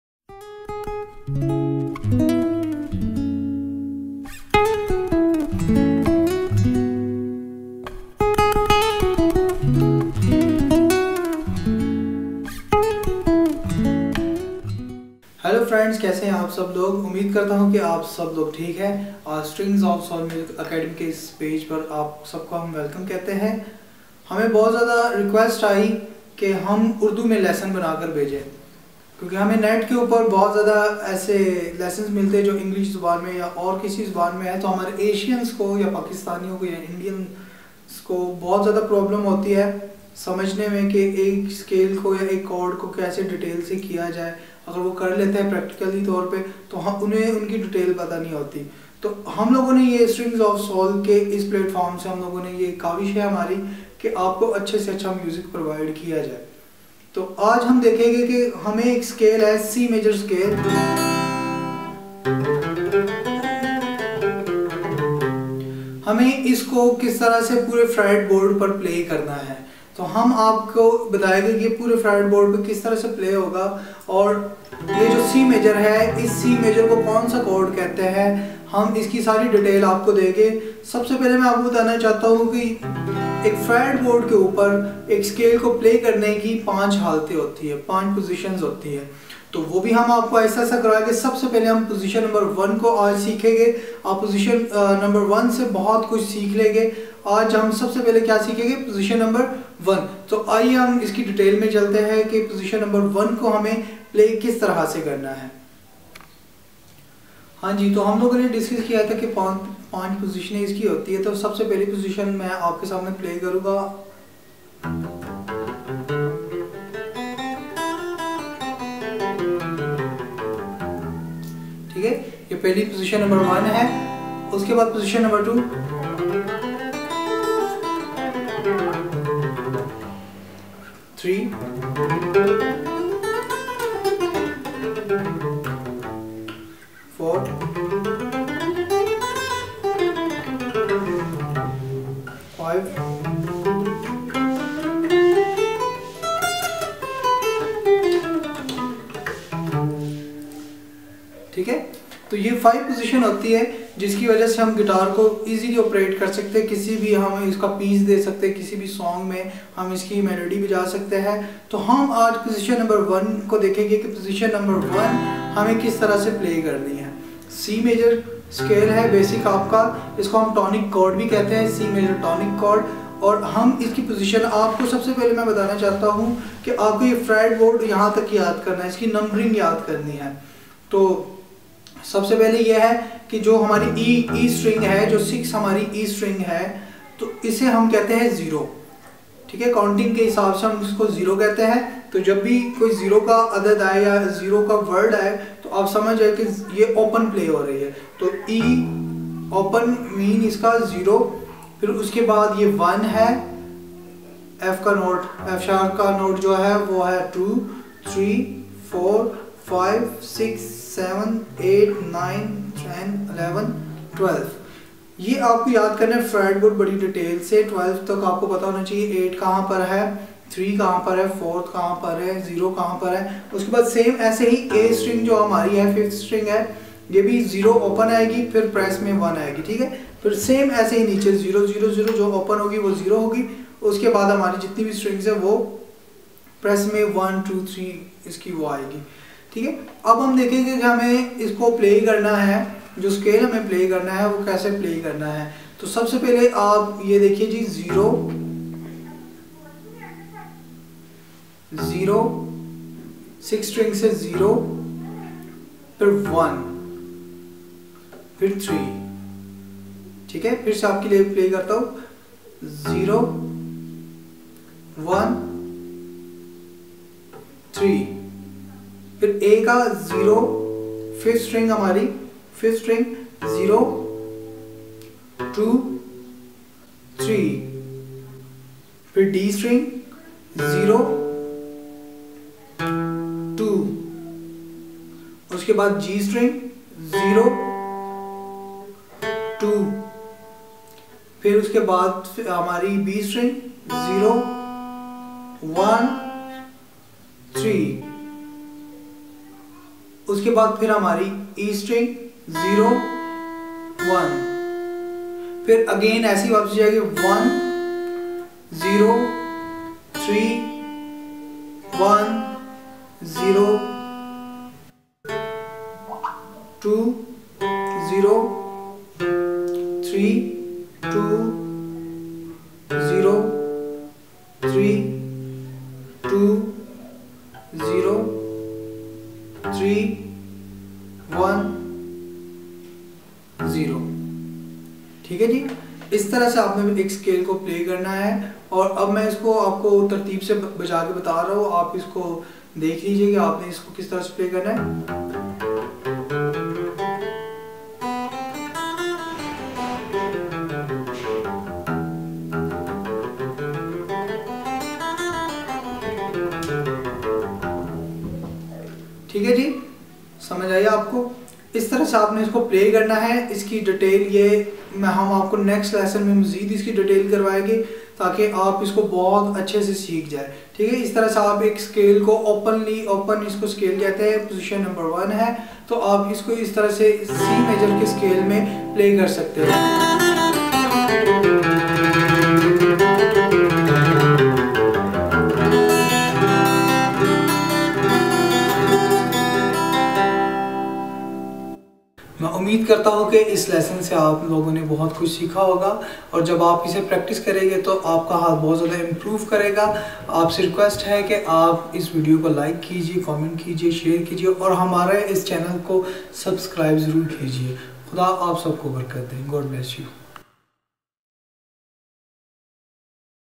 Easy C major scale position 01-lesson 01 in Hindi by faisal saddique
after doing this lesson,i hope your hand will be better on your hand